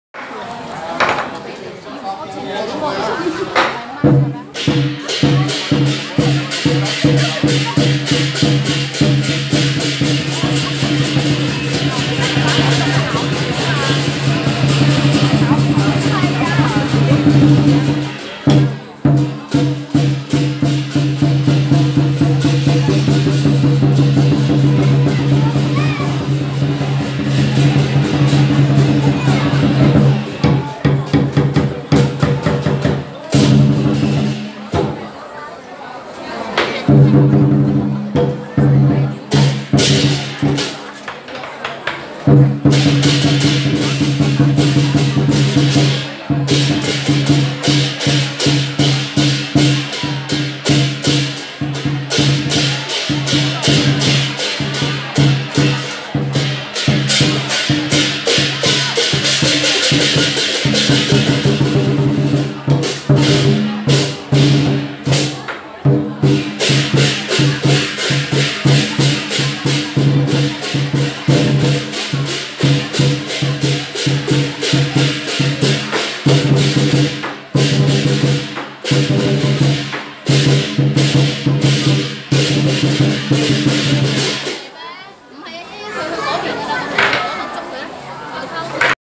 Chinese Tradition
Dragon Visiting every stablishements for Good luck